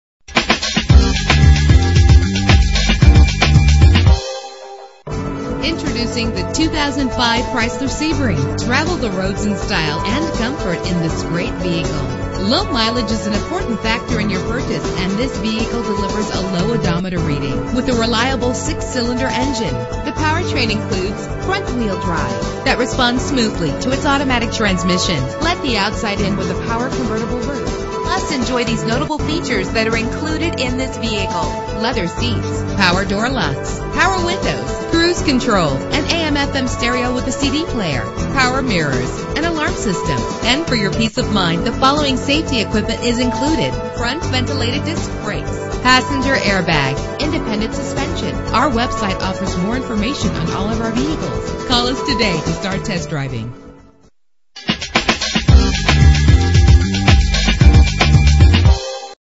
2005 Chrysler Sebring - Vestal NY
Year: 2005
Make: Chrysler
Model: Sebring
Trim: Touring
Engine: 2.7 liter 6 cylinder 24 valve
Transmission: 4-Speed Automatic
Color: Silver
Mileage: 0
Address:
3721 Old Vestal Rd